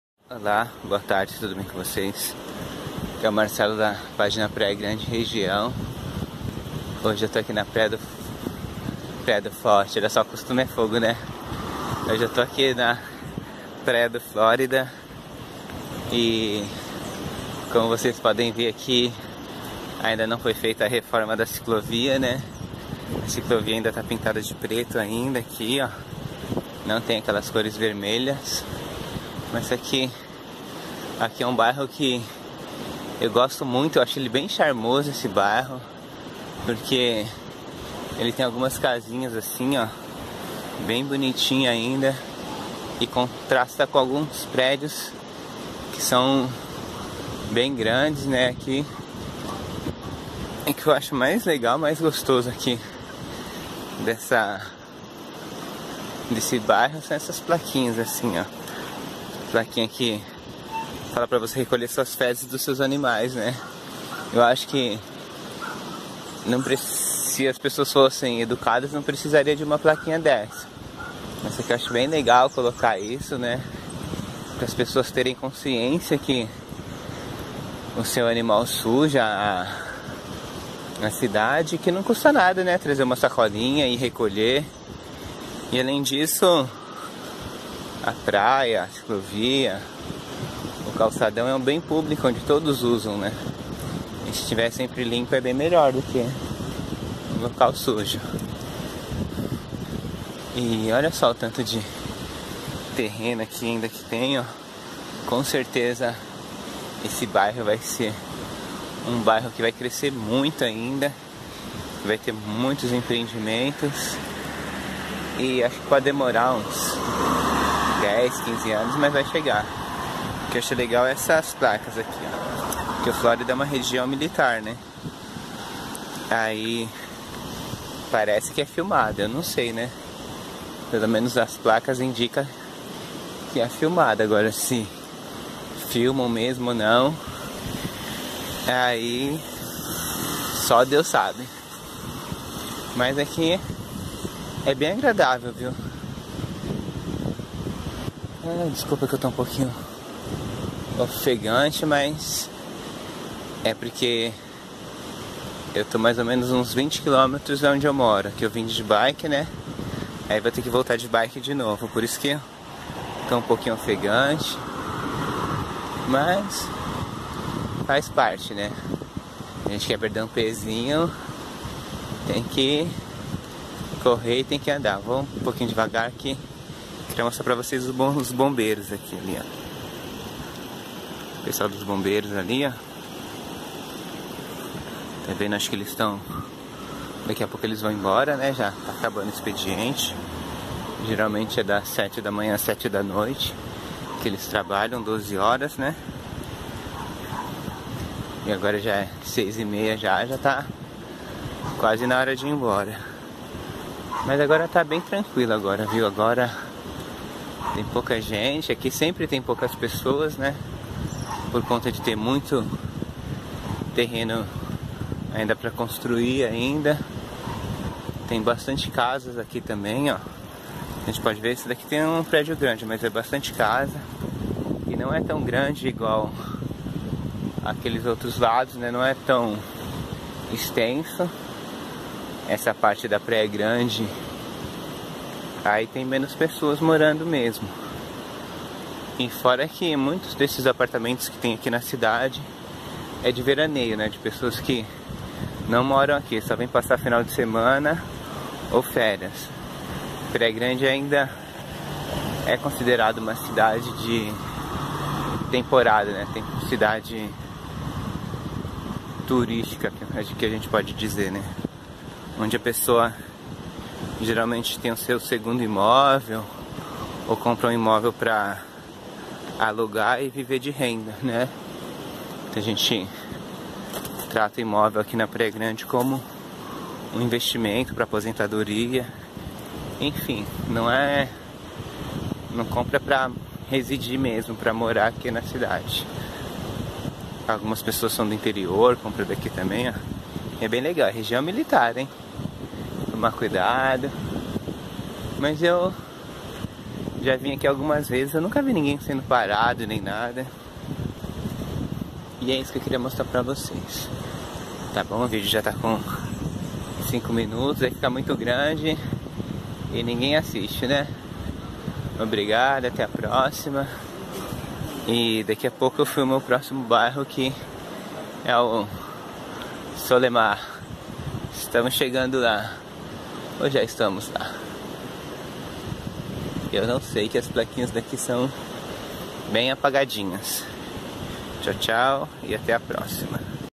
Ciclovia e calçadão na Praia Grande, Bairro do Real e Balneário Flórida
Telefone de contato Wharshapp (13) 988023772
Conecte-se com minha página Praia Grande e Região.
Olá boa noite tudo bem com vocês, aqui é o Marcelo da pagina Praia Grande e região e essa é a ciclovia e calçadão do bairro do real e Flórida, nas placas dizem que essa região é filmada e você pode ser parado a qualquer momento, mas eu nunca fui parado, alguém que mora no Flórida ou no Real já foi parado ???